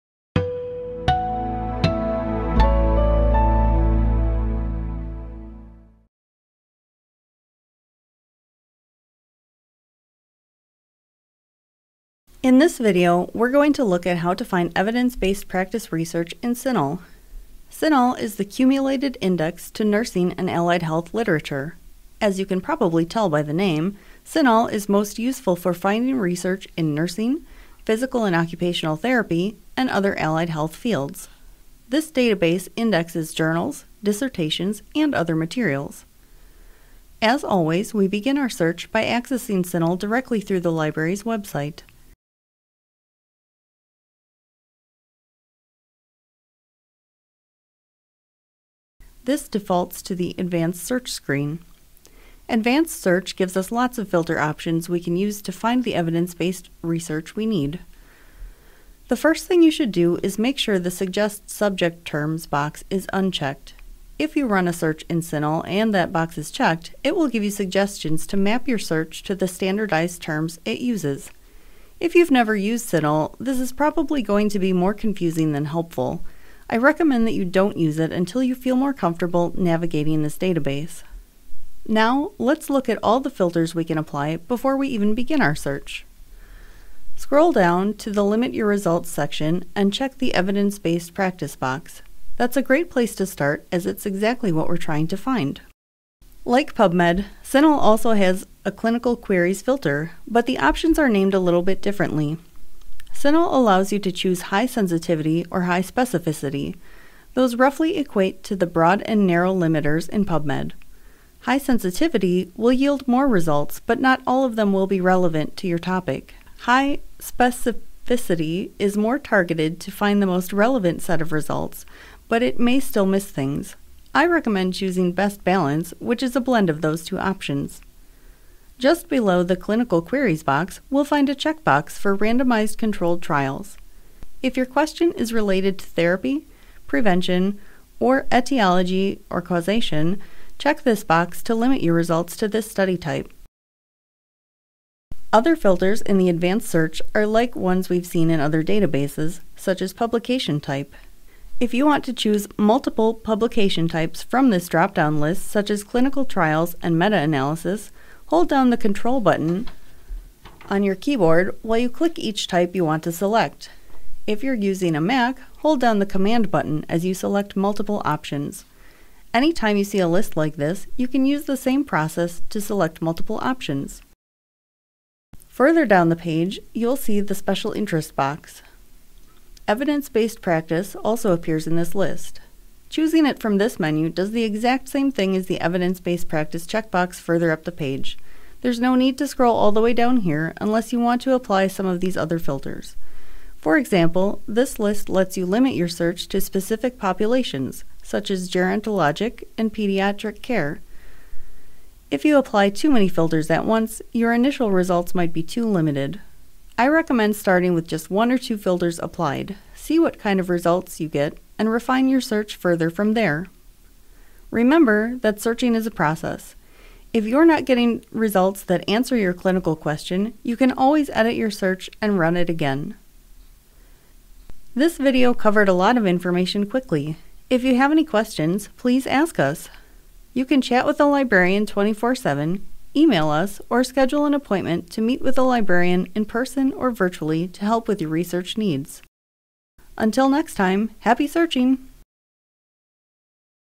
Introduction to CINAHL
This brief tutorial will demonstrate how to search CINAHL the Cumulated Index to Nursing and Allied Health Literature. CINAHL is most useful for finiding research in nursing, physical/occupational therapy, and other allied health fields. This database indexes journals, dissertations, and other materials.